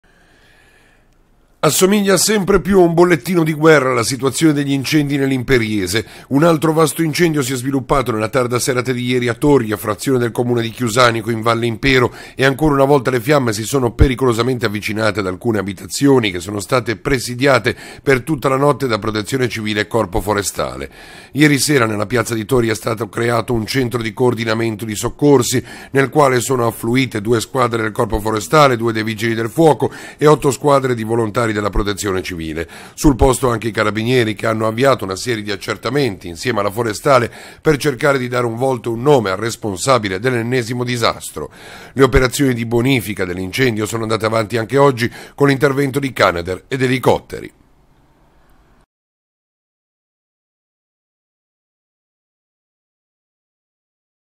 INCENDIO BOSCHIVO A TORRIA
Anche ieri sera i boschi dell’entroterra imperiese sono tornati a bruciare. Questa volta è toccato alle alture di Torria, frazione del comune di Chiusanico, in Valle Impero e le fiamme si sono di nuovo avvicinate ad alcune case. Le operazioni di bonfica sono andate avanti anche oggi